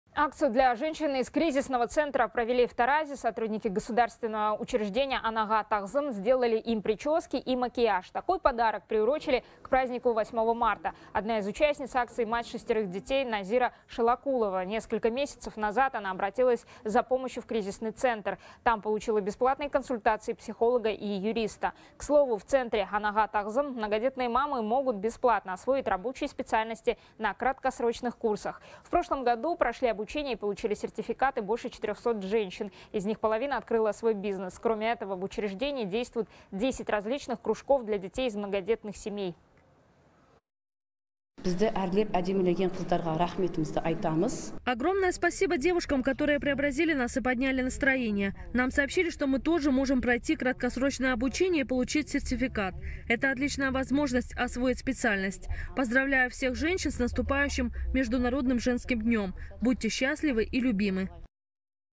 Акцию организовали для женщин из кризисного центра
Акцию для женщин из кризисного центра провели в Таразе, передает «24KZ». Сотрудники государственного учреждения «Анаға тағзым» сделали им причёски и макияж. Такой подарок приурочили к празднику 8 Марта.